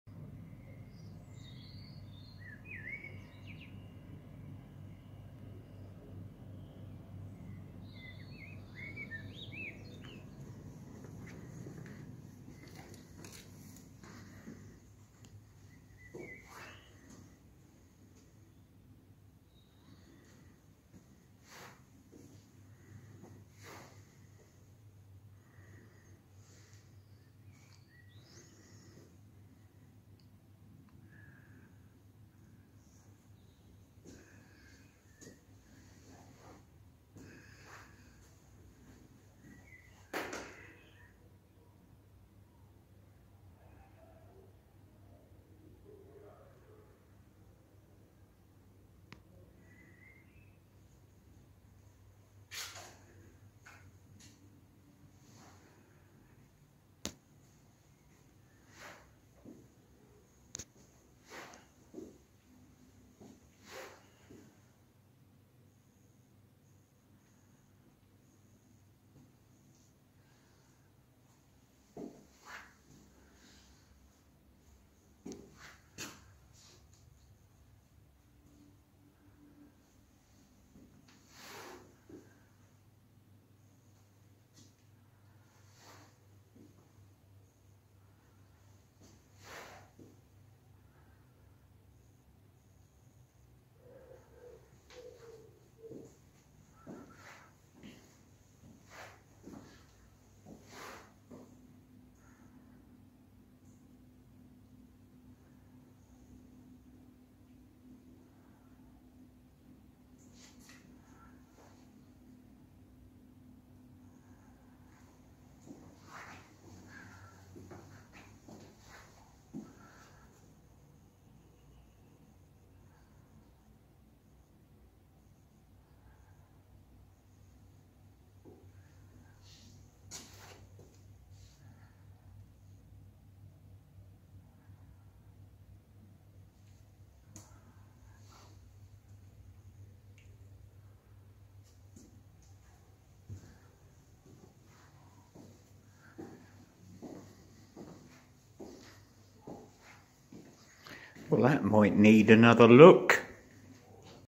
Chris Newson Art gallery.
2 minute film showing some of his work